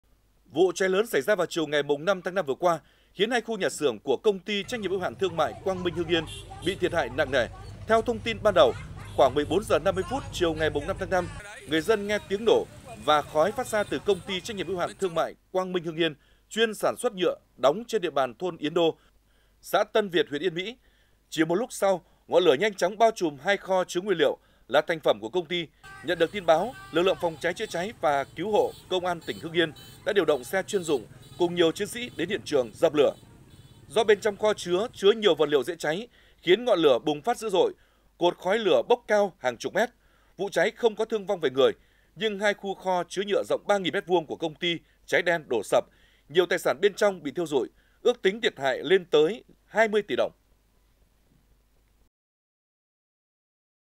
Cháy tại Công ty Quang Minh Hưng Yên gây thiệt hại trên 20 tỉ đồng
Hưng Yên TV | Cháy tại Công ty Quang Minh Hưng Yên gây thiệt hại trên 20 tỉ đồng
Địa chỉ: 164 Nguyễn Văn Linh, thành phố Hưng Yên, tỉnh Hưng Yên
Tiktok: Truyền hình Hưng Yên
Facebook: Đài Phát thanh và Truyền hình Hưng Yên
Zalo: Đài PTTH Hưng Yên